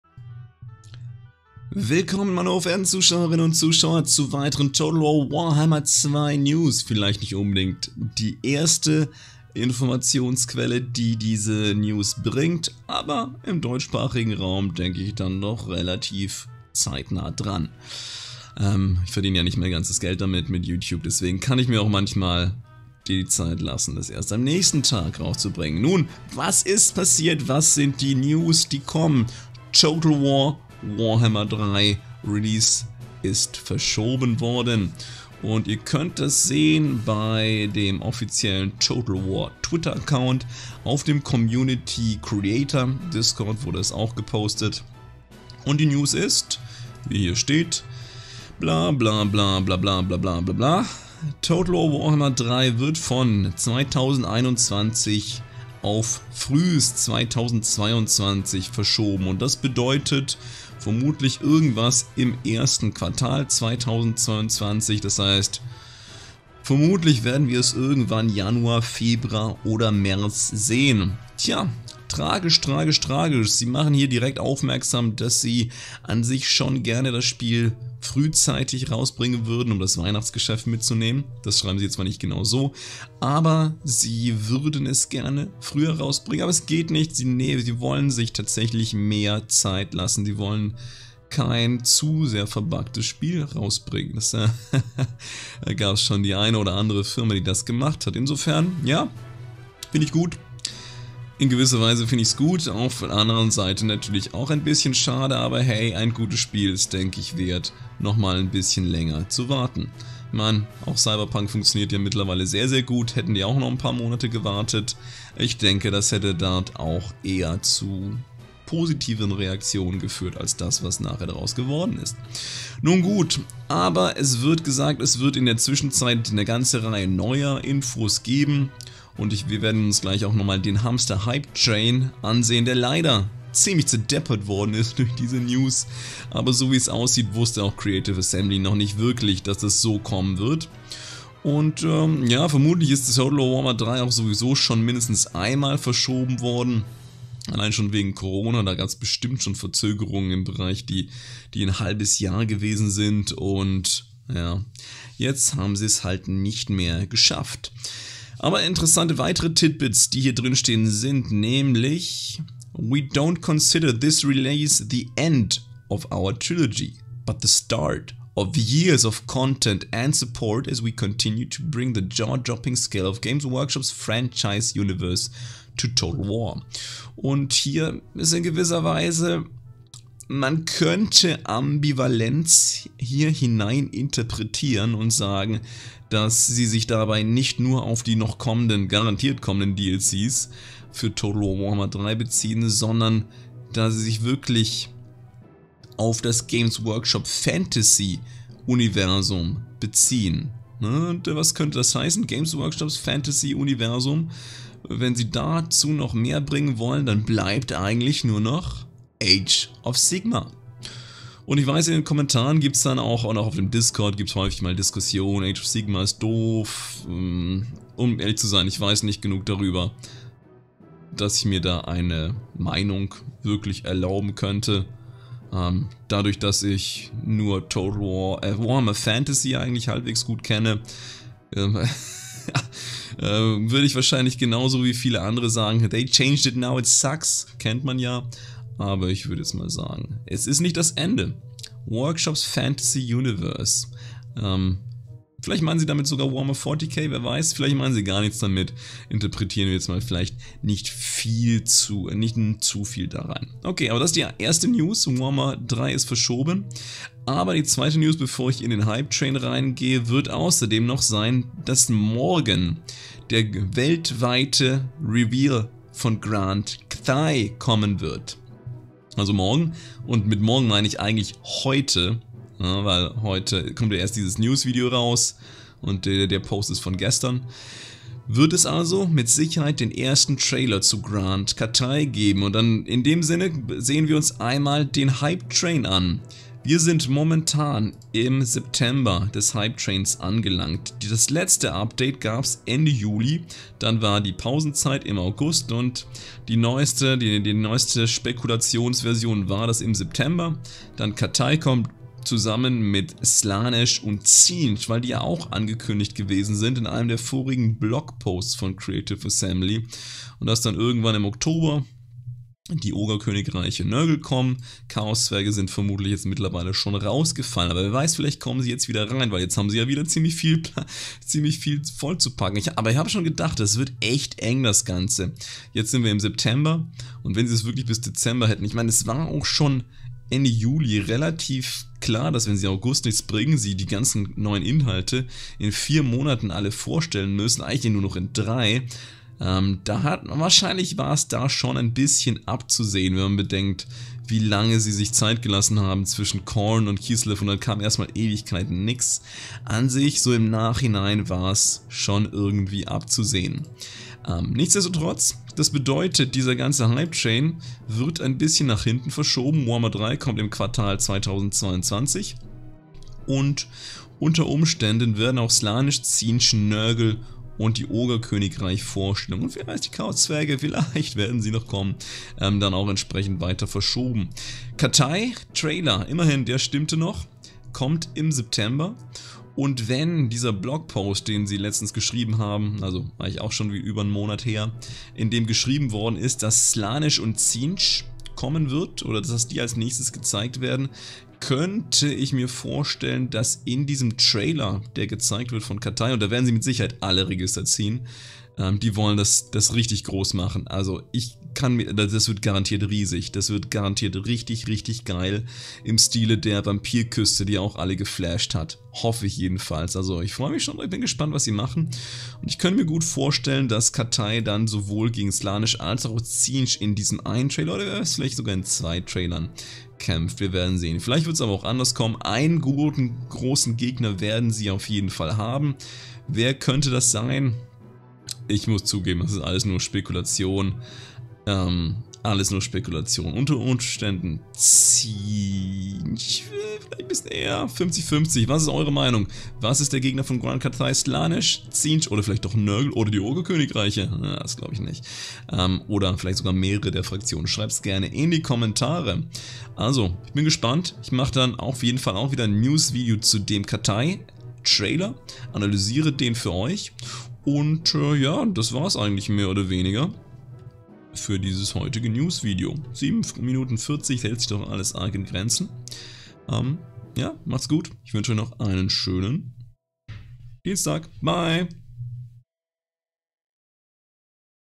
Warhammer 3 VERSCHOBEN - CATHAY REVEAL HEUTE - Total War: Warhammer 3 NEWS
Die gezeigten Spiele sind Eigentum der jeweiligen Produzenten und Veröffentlichern.